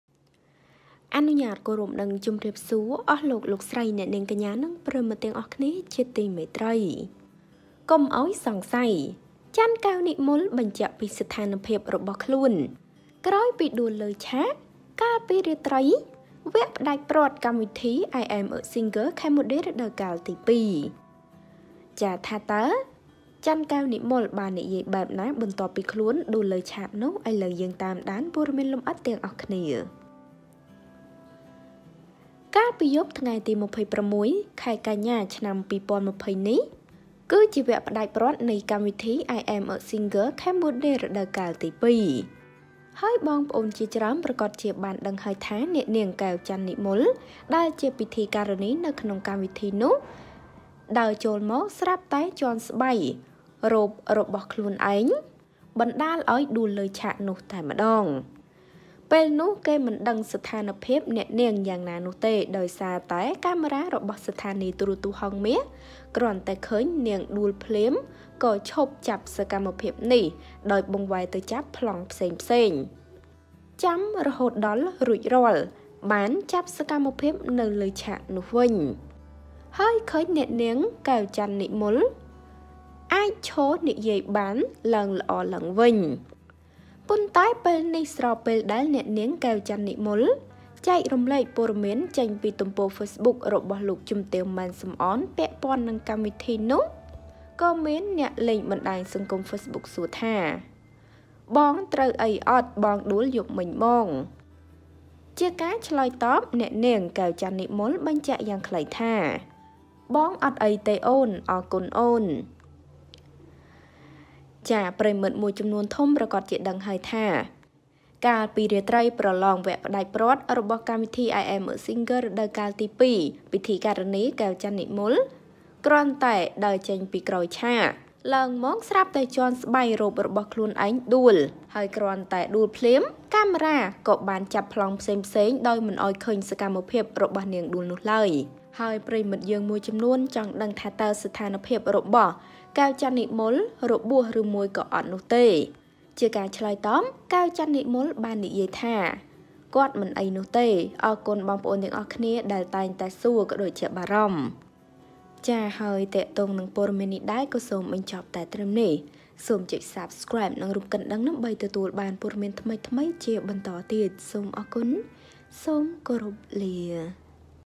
កុំអោយសង្ស័យ! ចាន់ កែ​និមល បញ្ជាក់​ពី​ស្ថានភាព​របស់ខ្លួន ក្រោយពី​ដួល​លើ​ឆាកយប់មិញ មួយម៉ាត់យ៉ាងខ្លីថា
សូមអធ្យាស្រ័យ

ចំពោះការមិនបានតប ខំមិនបានគ្រប់គ្នា

Ms Socheata